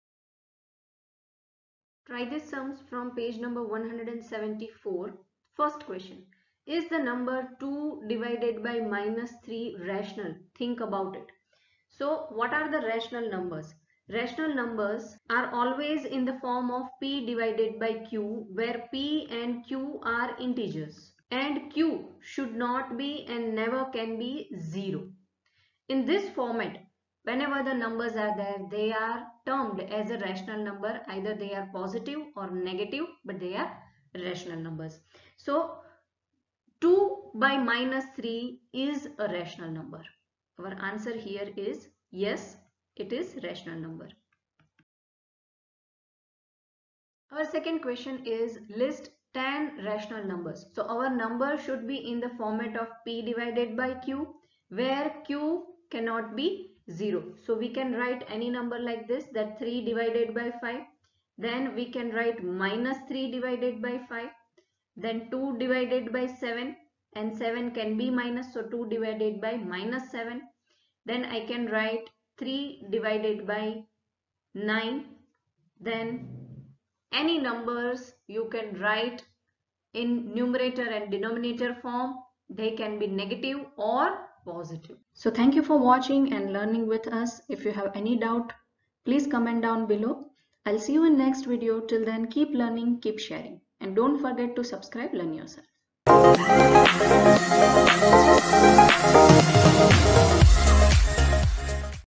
Try These page no. 174 chapter-9|| CBSE Std-7 Math Ch-9|| NCERT class-7
1. Is the number 2/−3  rational? Think about it.
2. List ten rational numbers